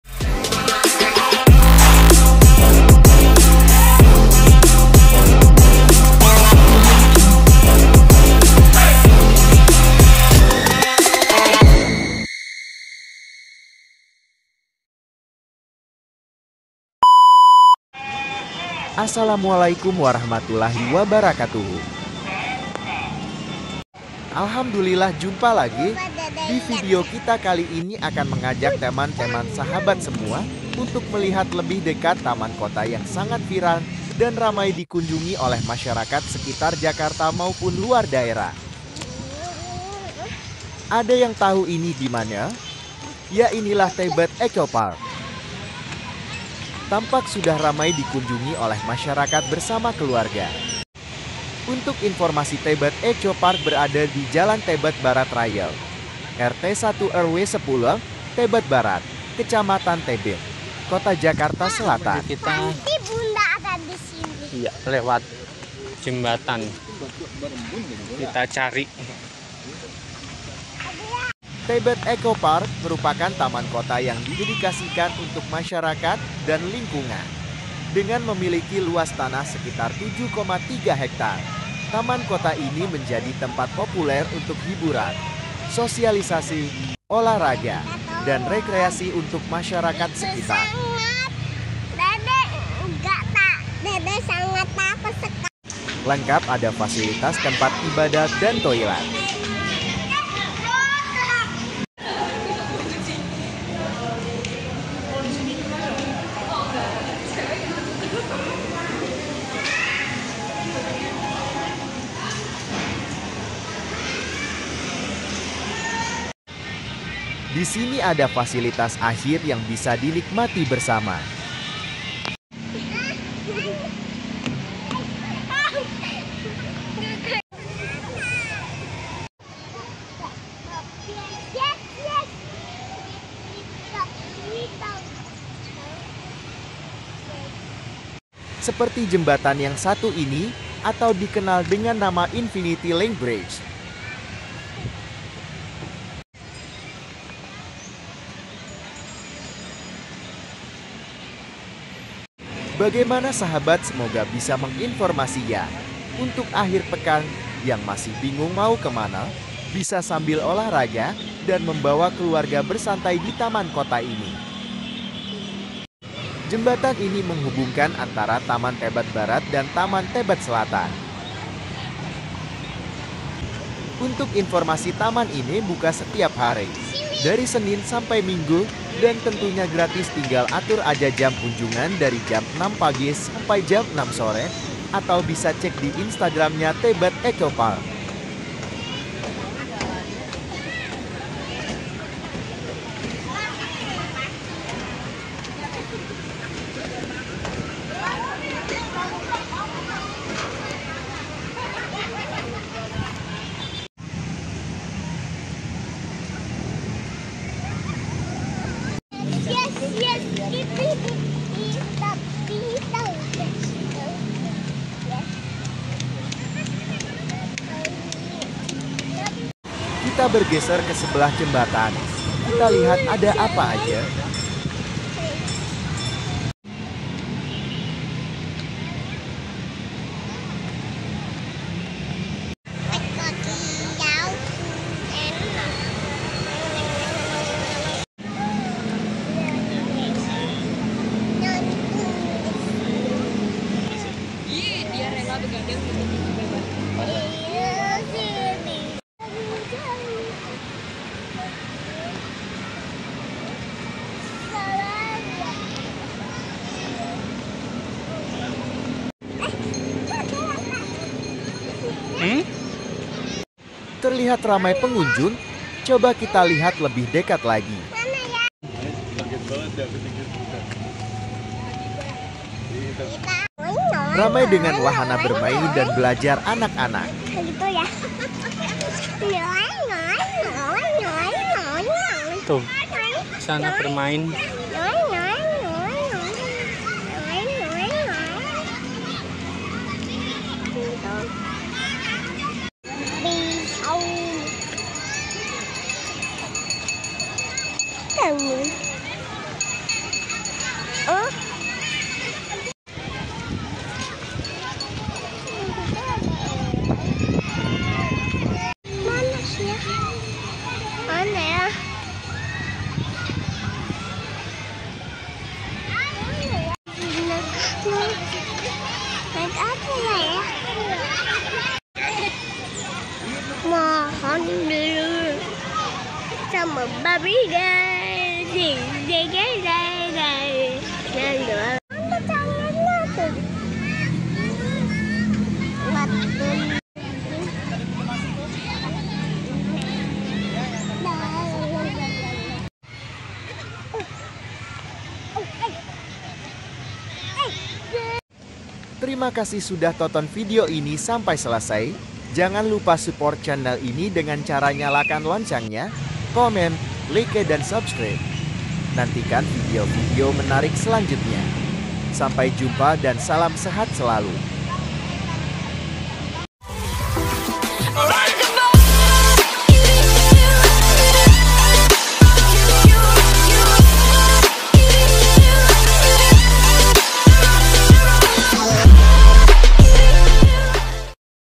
SERUNYA MENGHABISKAN LIBUR PANJANG TEBET ECO PARK!! GRATIS dan INSTAGRAMABLE
Assalamualaikum warahmatullahi wabarakatuhu. Tebet Eco Park (TEP) merupakan taman kota yang didedikasikan untuk masyarakat dan lingkungan. Terletak di Jakarta Selatan dengan area seluas 7,3 hektare.

sarana rekreasi keluarga, ruang terbuka Hijau, olahraga, sosialisasi, dan tempat untuk hiburan.